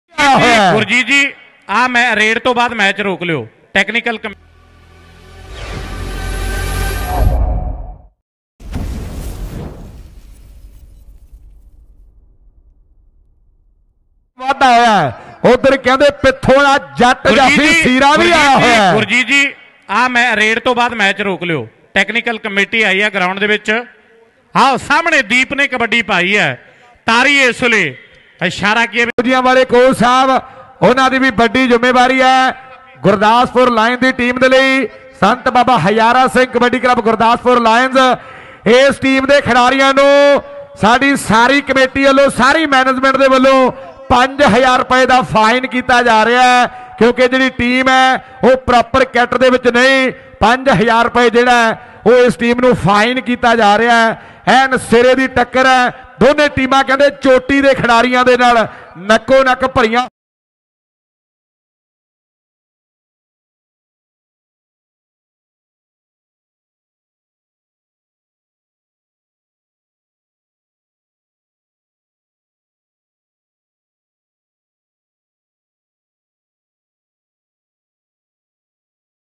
ਅੱਜ ਦਿੱਲੀ ਵਿਚ ਗੁਰਦਾਸਪੁਰ ਲਾਈਨਜ਼ ਨੂੰ ਹੋਇਆ ਜੁਰਮਾਨਾ | Delhi Morcha Kabaddi League | Day 3 Tikri Border
ਅੱਜ ਦਿੱਲੀ ਵਿਚ ਗੁਰਦਾਸਪੁਰ ਲਾਈਨਜ਼ ਨੂੰ ਹੋਇਆ ਜੁਰਮਾਨਾ | Delhi_Morcha_Kabaddi_League | Day 3 Tikri_Border | Kabaddi Live 2021